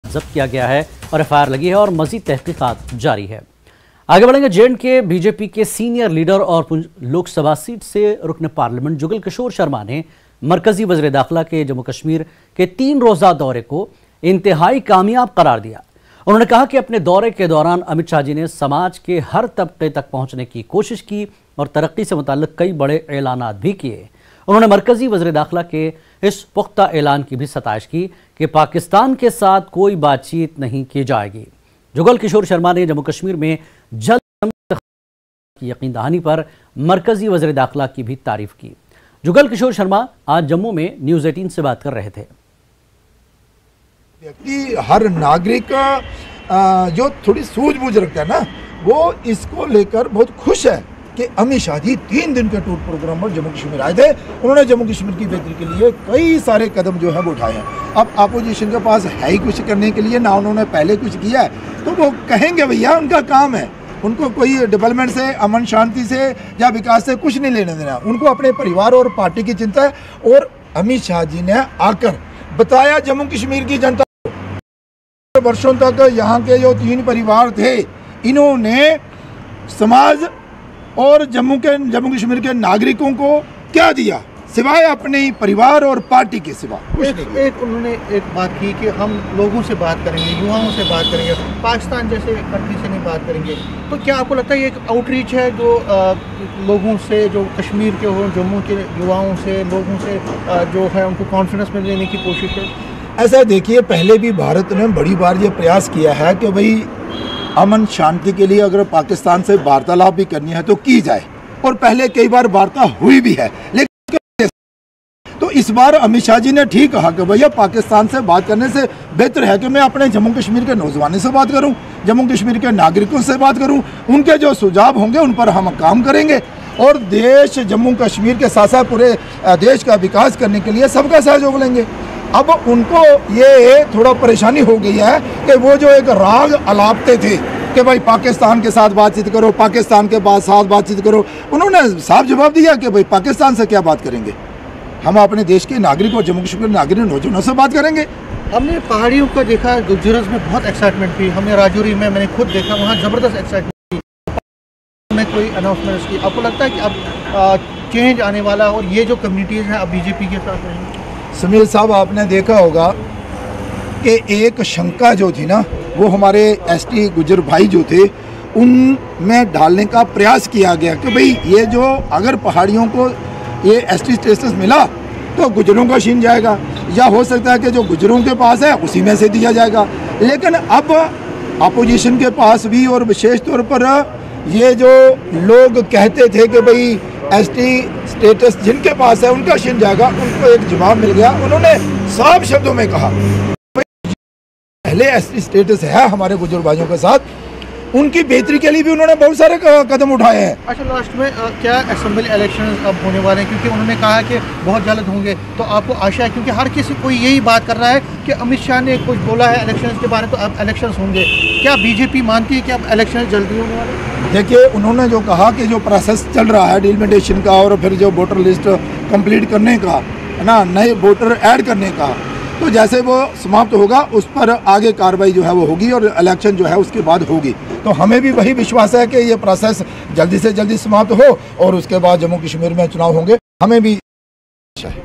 Kashmir News l Jugal Kishore Sharma Ne Amit Shah Ki Rally Ko Kamyab Qarar Diya l News18 Urdu
In his interaction with News18 Jugal Kishore Sharma said that the rally of Amit Shah is a grand success l n18oc_Jammu